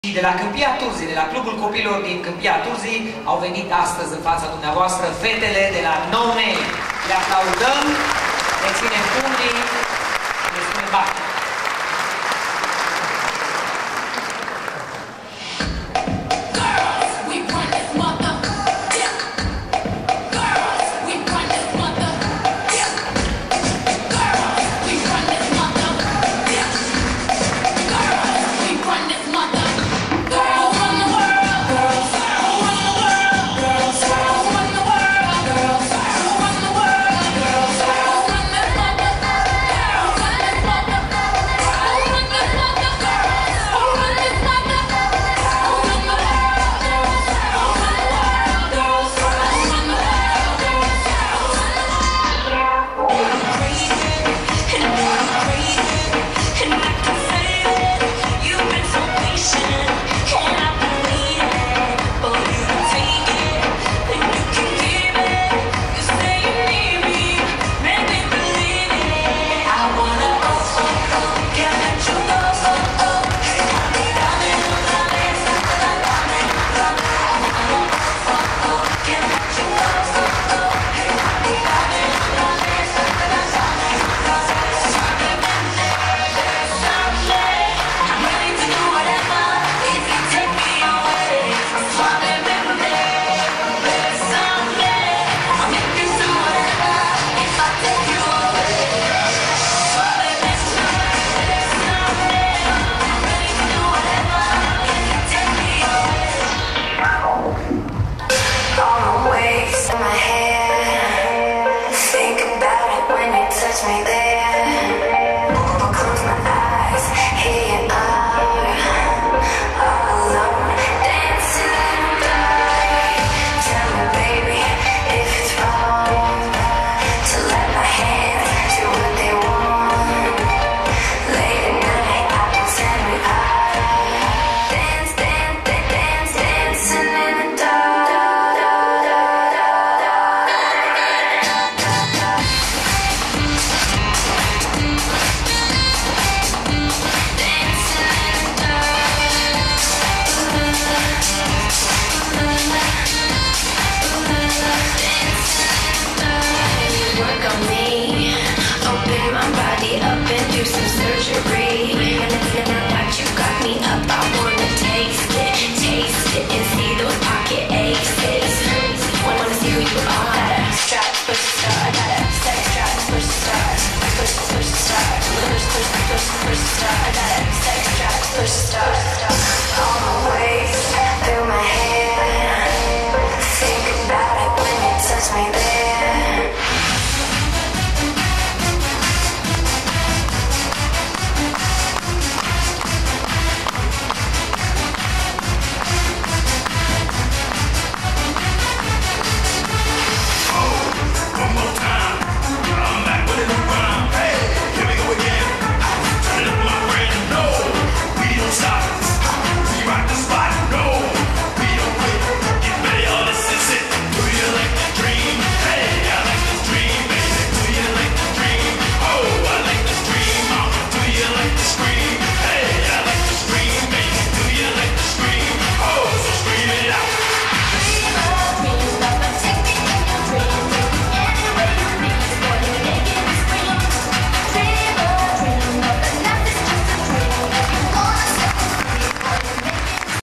Majoretele No Name Clubul Copiilor Campia Turzii
La Campionatul Judetean de Majorete Cluj 2012